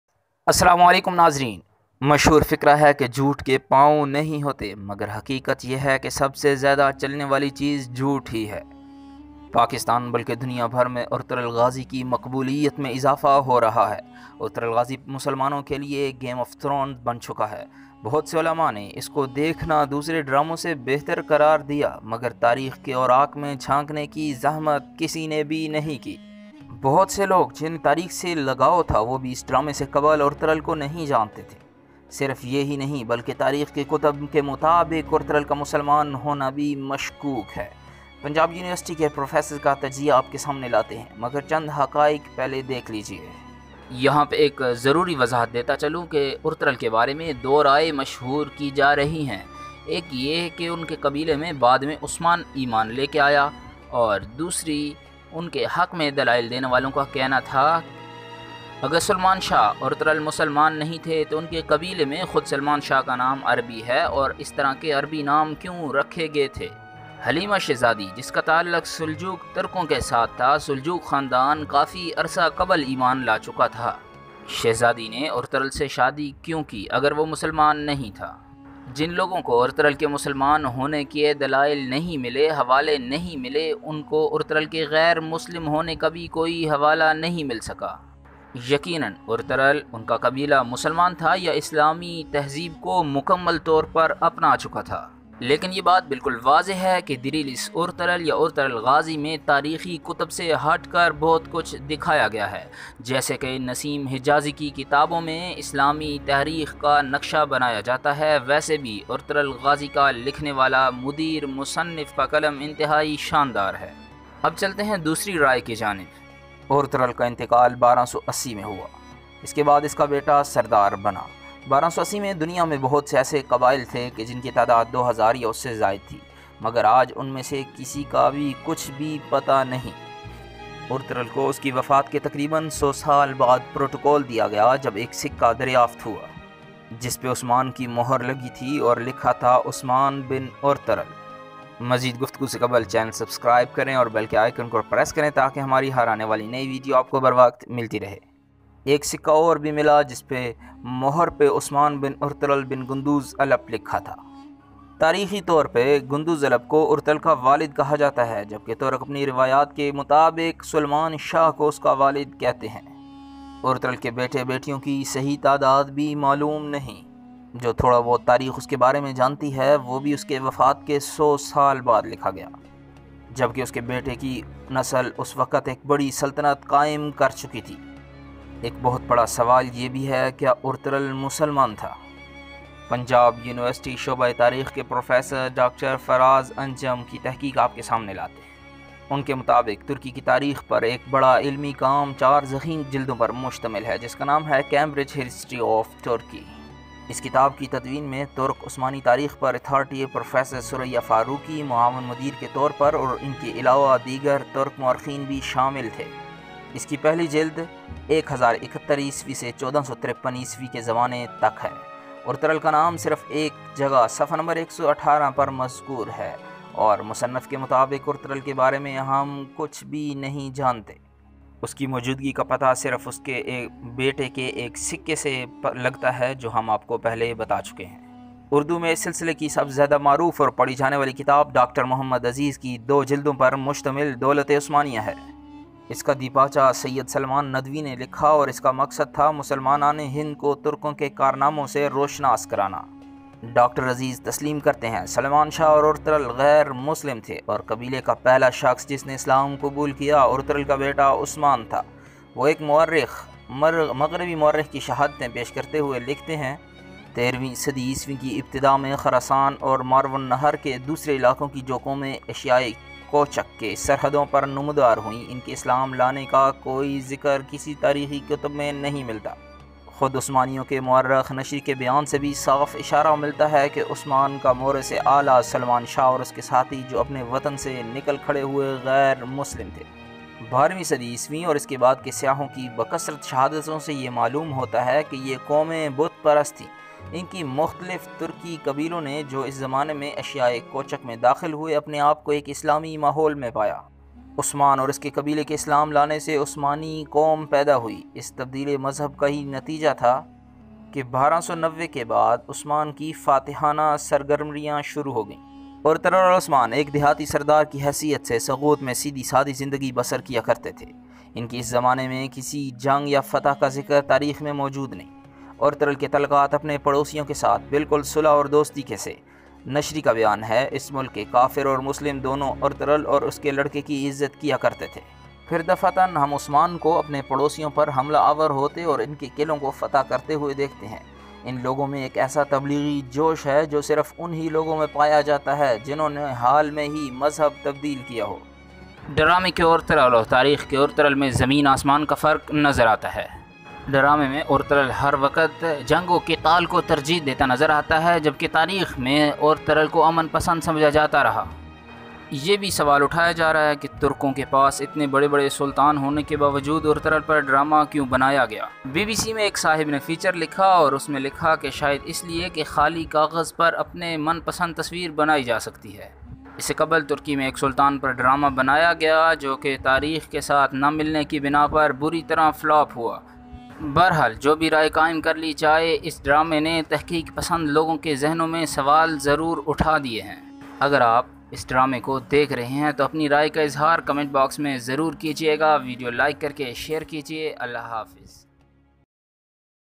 Was Ertugrul Muslim or Non Muslim | True Story of Ertugrul Ghazi | ارطغرل غازی غیر مسلمان تھا؟ | ASI
The Reality of Ertugrul Ghazi...

Was Ertugrul Gazi Muslim or Non Muslim...?